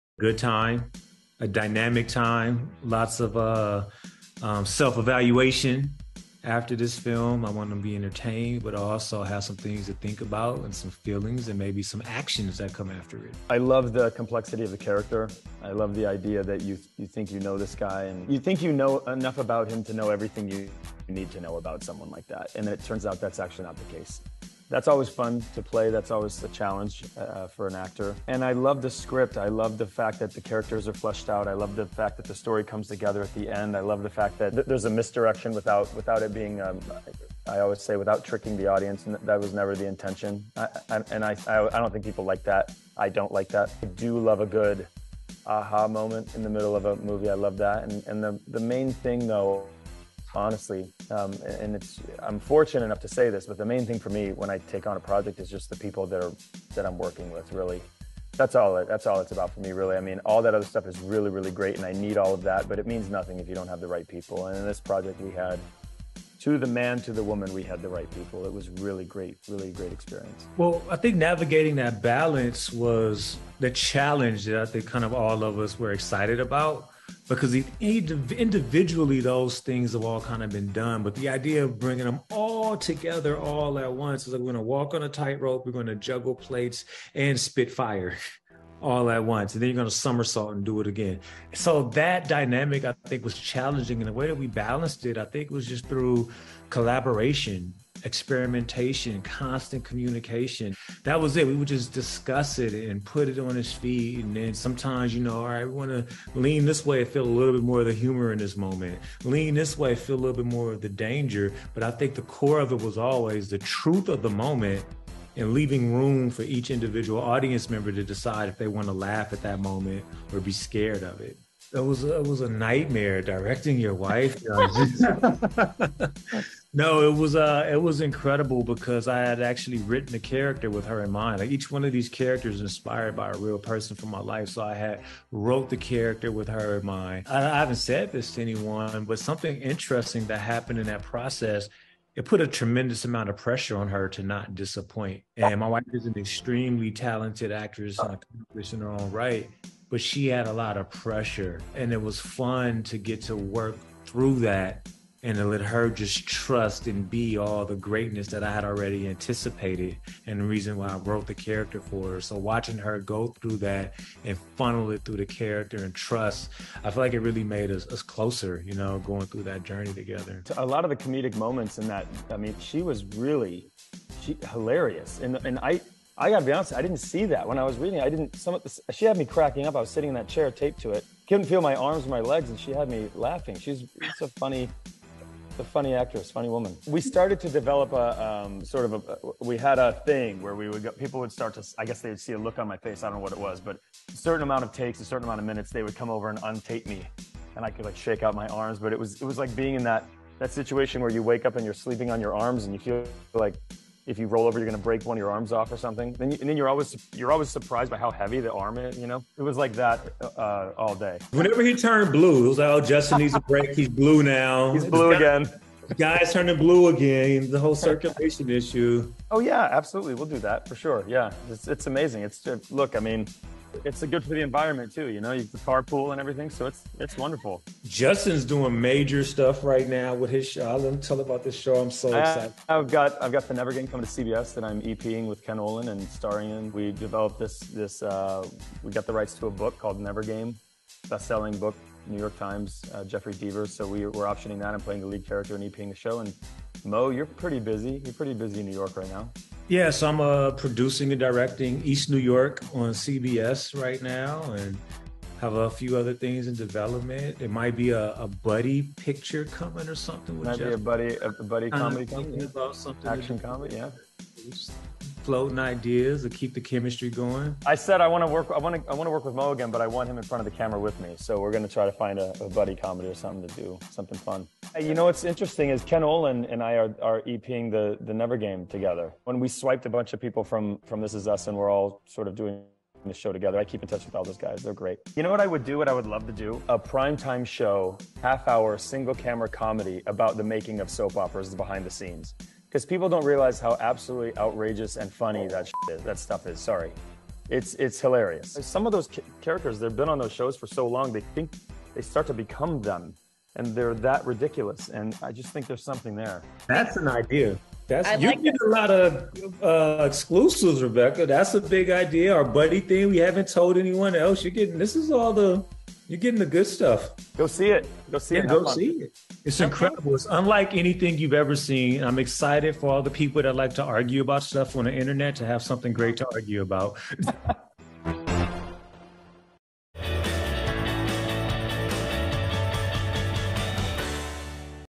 Justin Hartley and Mo McRae Discuss New Movie & Tease MAJOR Projects | E! Insider
Justin Hartley details the complexities of his character in Mo McRae's film, "A Lot of Nothing." Plus, the "This Is Us" alum gives scoop on new CBS show and more!